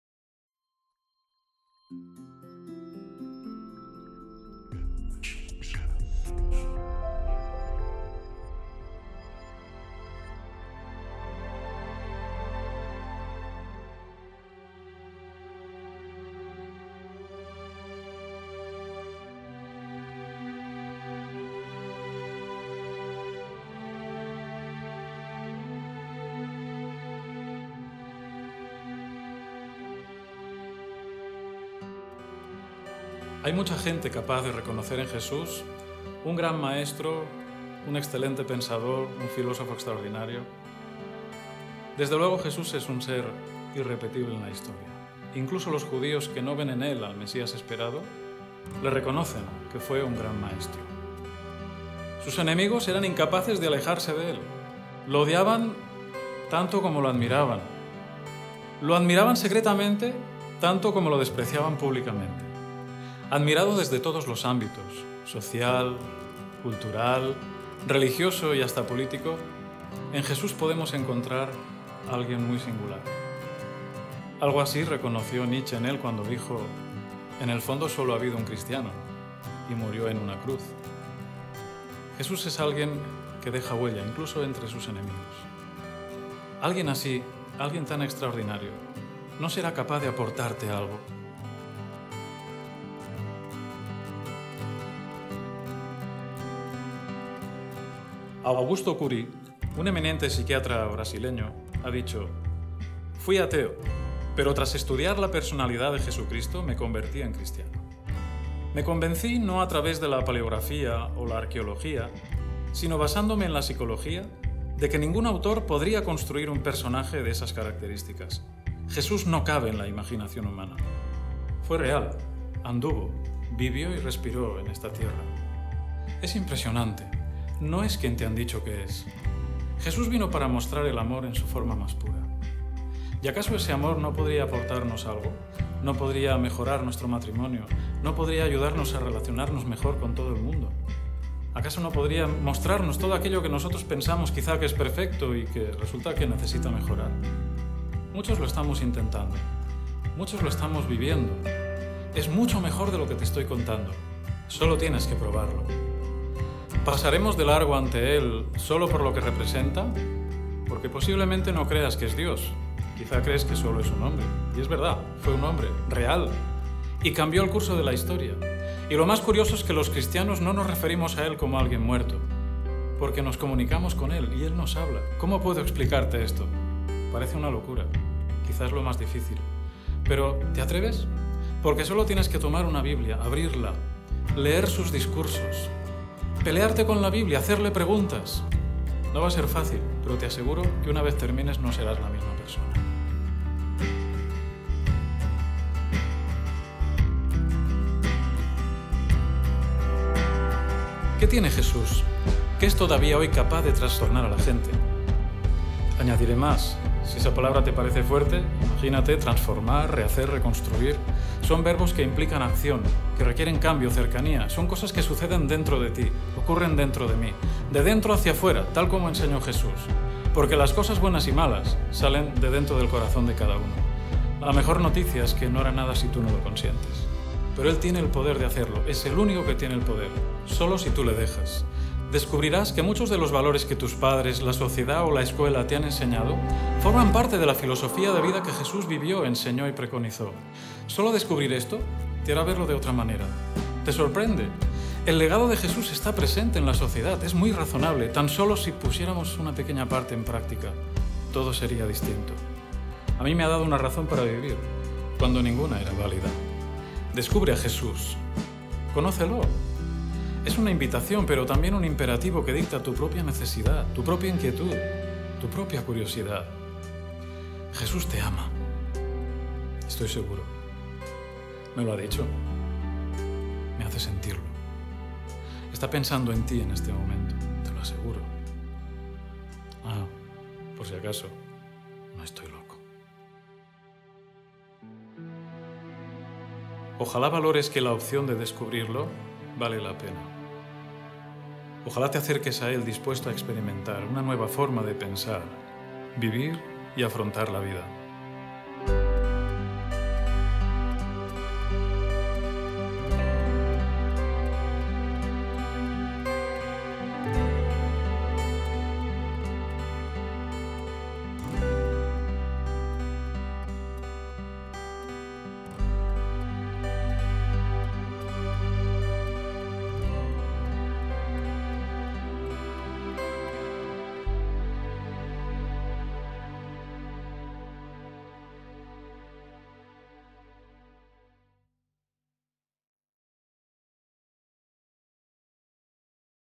YO PENSABA QUE... 04 Rewind [Completa - Canal Oficial]
04 Rewind - Carlos Catalán
No es quien te han dicho que es. No cabe en la imaginación humana. Fue real, anduvo, respiró y vivió en esta tierra. Sus enemigos eran incapaces de alejarse de él, lo odiaban tanto como lo admiraban; lo admiraban secretamente, tanto como lo despreciaban públicamente. Alguien así, alguien tan extraordinario, ¿no será capaz de aportarte algo?